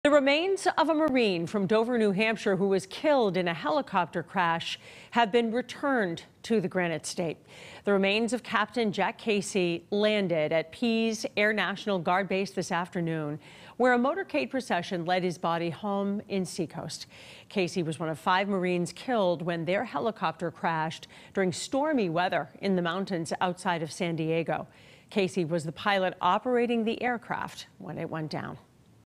Procession honors New Hampshire Marine killed in helicopter crash
Marine Capt. Jack Casey, who was killed in a helicopter crash in California, returned home to Dover, New Hampshire on Tuesday.